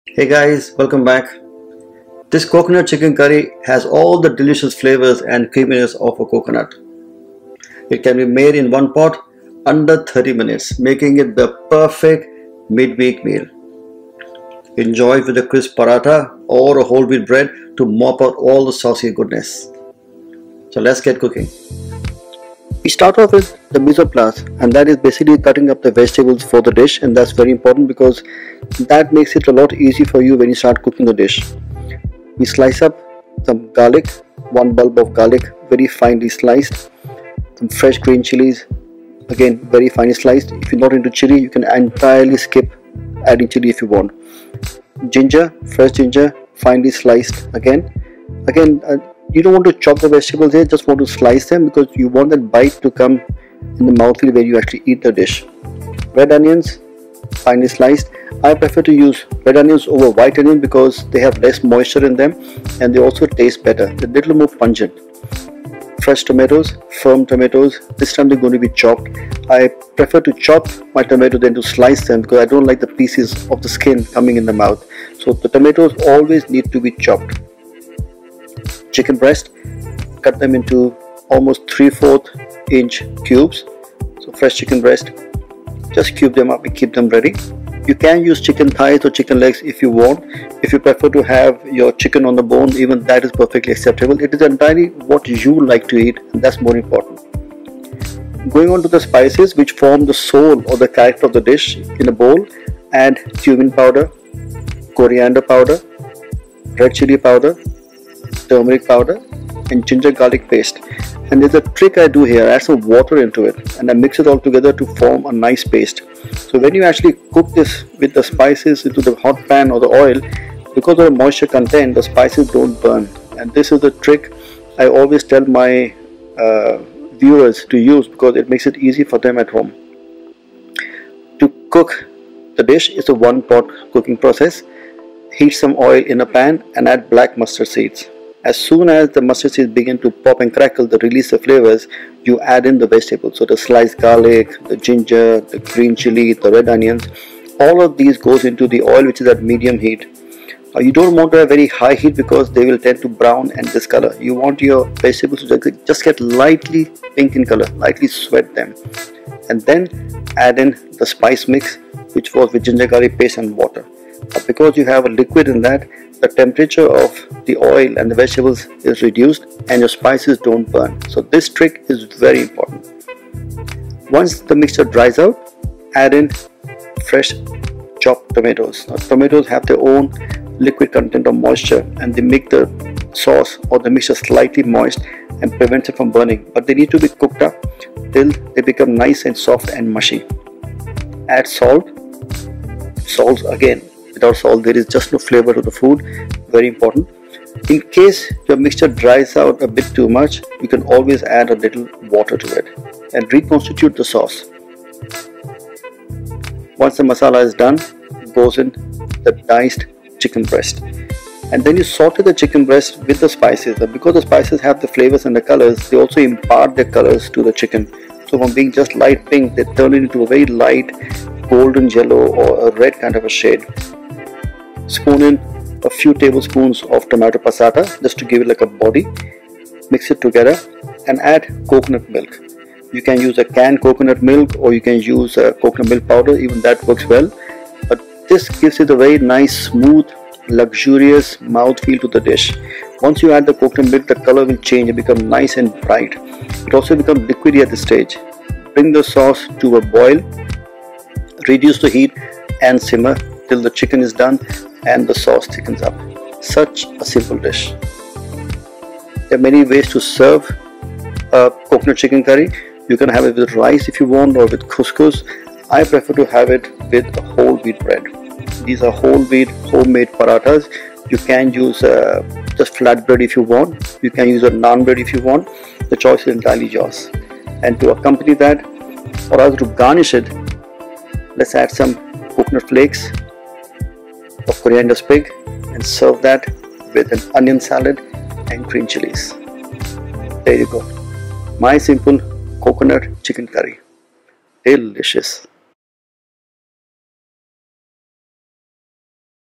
How To Make The BEST Chicken Curry (COCONUT CHICKEN CURRY)
COCONUT CHICKEN CURRY

40g vegetable oil
2 tablespoon mustard seeds
1 red onion, medium sized, finely sliced
1 clove garlic, sliced
1 small green chilli, sliced
1/2 inch cube ginger, sliced
2 tablespoon ginger and garlic paste
1 teaspoon red chili powder
1 teaspoon turmeric Powder
1 tablespoon coriander powder
1 tablespoon cumin powder
1 tomatoes, medium sized, finely chopped
5 tablespoon tomato passata
2 chicken breast, boneless & skinless, cut into 1 inch cubes
200ml coconut milk, thick

Method:
Heat oil in a thick deep bottomed pan; add the mustard seeds and as soon as it crackles add the sliced garlic, ginger, green chillies and  red onion, sauté the onions until golden brown.

Mix together red chili powder, coriander powder, cumin powder, turmeric powder & ginger garlic paste along with enough cold water to soak the mixture. Add this to the onion mixture and cook for a minute.

Now add chopped tomatoes and sauté occasionally muddling them till it is a smooth paste, add the diced chicken. Cook while stirring constantly for another 2 minutes.
Add tomato passata and continue cooking till the chicken is half cooked, add the coconut milk and salt.
Bring the sauce to a boil, reduce heat and simmer till the chicken is fully cooked.

Serve hot with steamed rice or paratha.